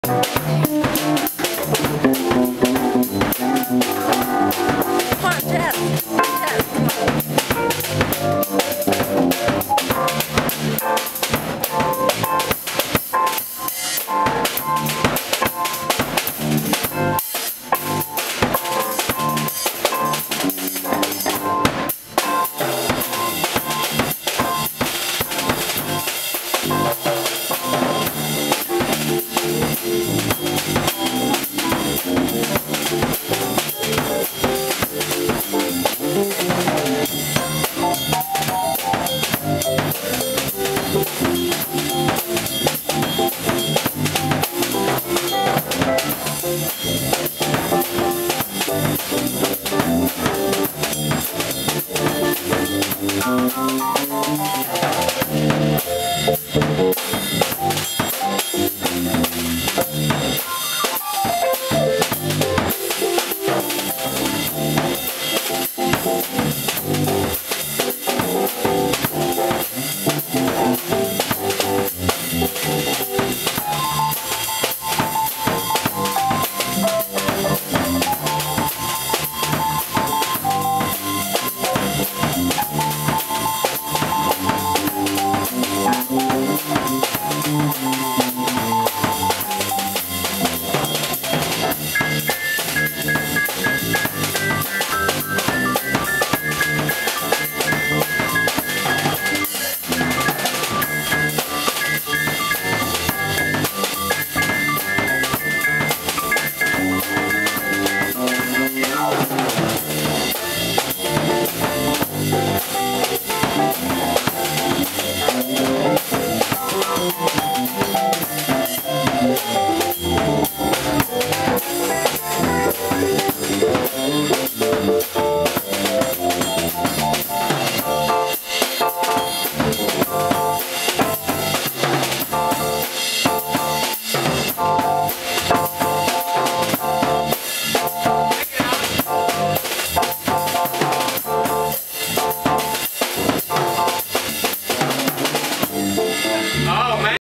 Groovin with Alex & Charlie Russell
Layin' down a groove with Charlie Russell, and 11 year-old phenom Alex Girshevich at our Christmas party.